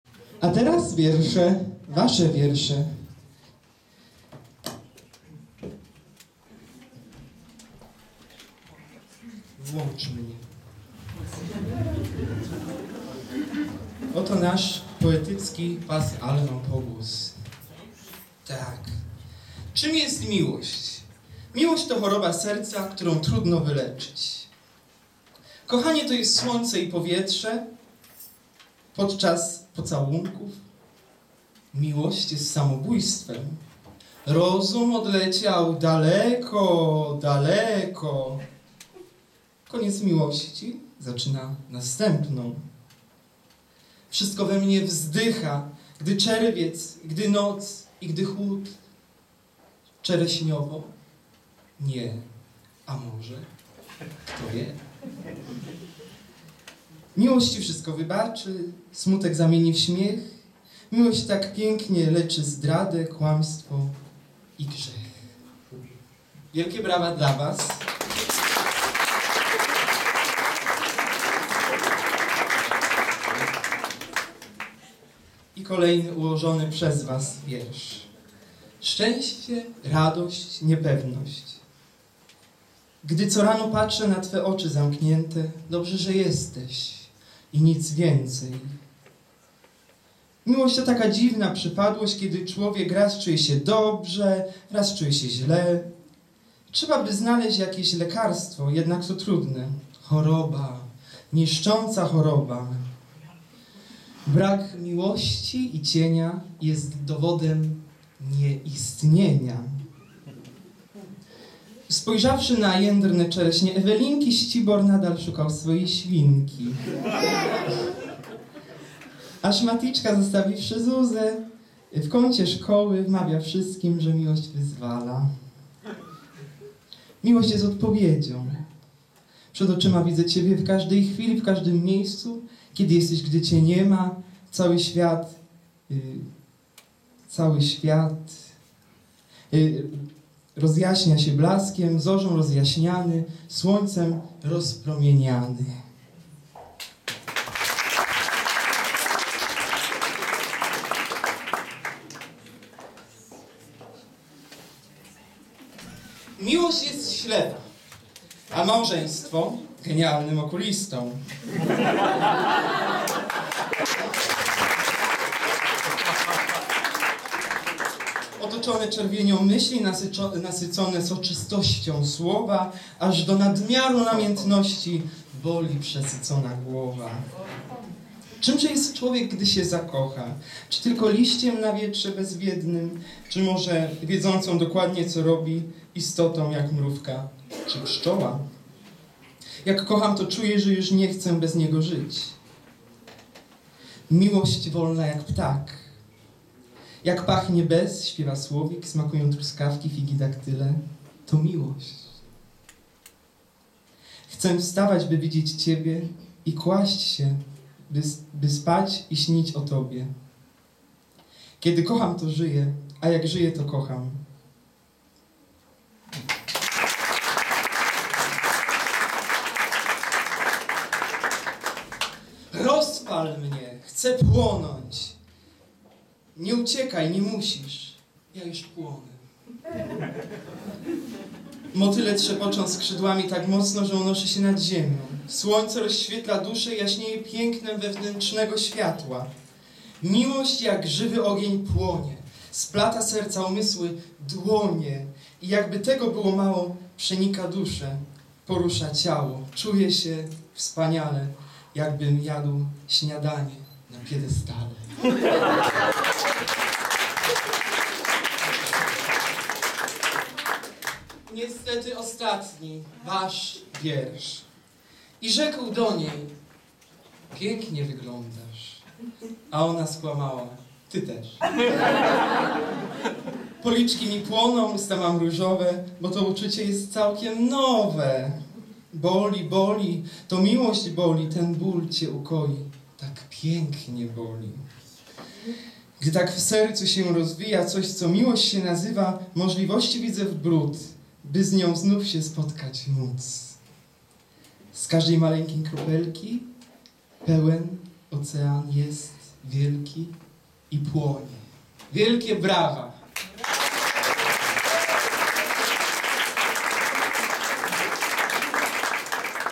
Artenground 3 - Wiersze Publicznosci - Pasjans poetycki
Artenground 3 - Pasjans z kart z poezją napisaną przez publiczność artengroundową.

Poezję czyta Szymon Bojdo.

Artenground wspiera agencja artystyczna Jazz Event oraz Schron Kultury Europa.